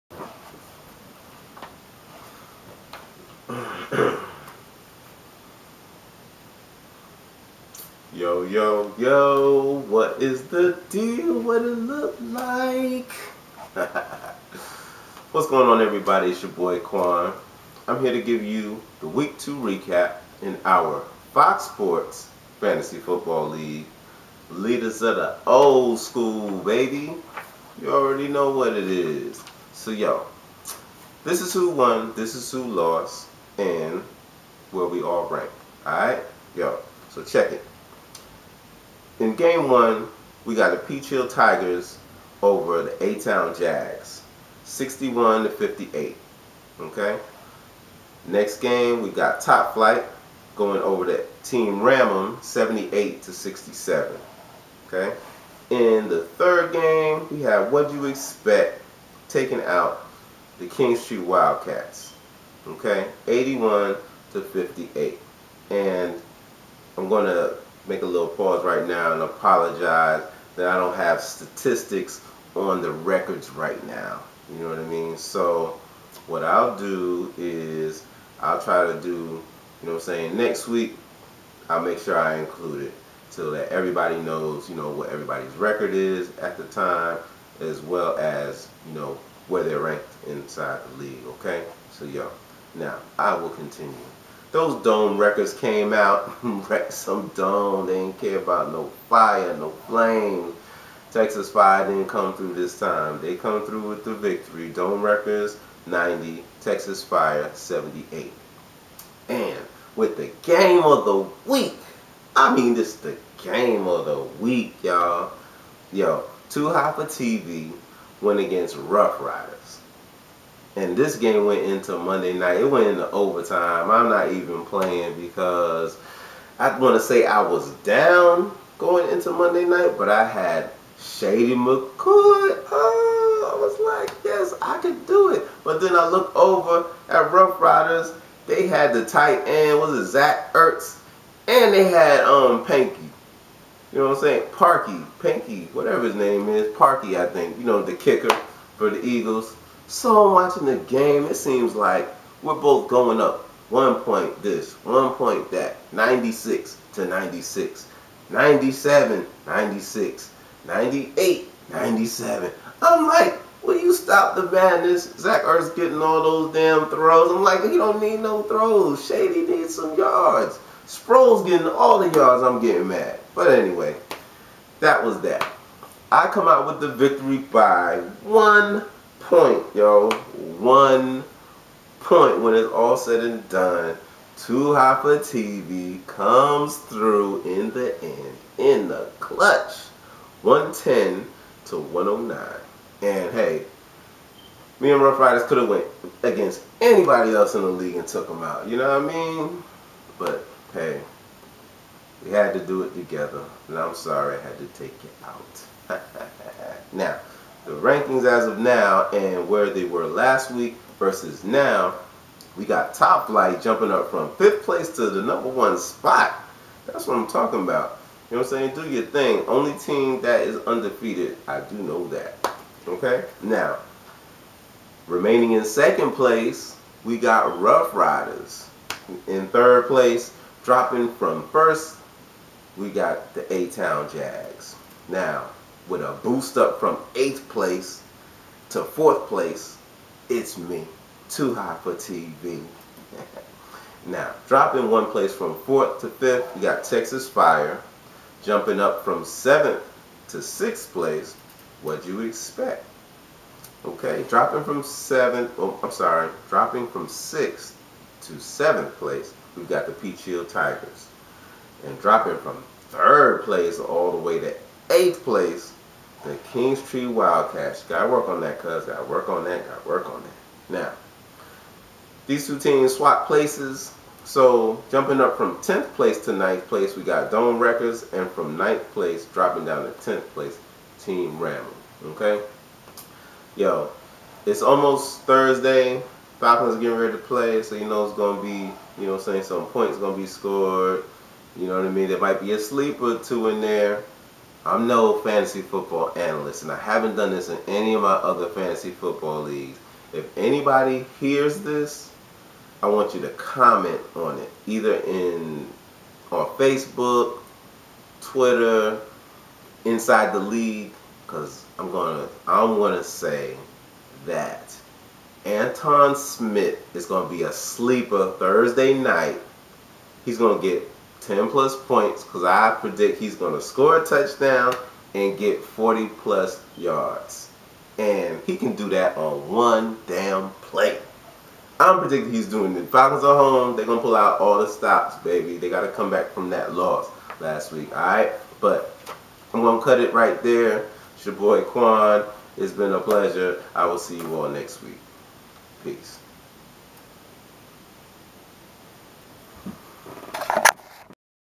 Leaders of the Old Skool - Week 2 Recap - FOXsports Fantasy Football
Did some teams make a comeback? Did some remain undefeated? You've gotta watch it to see!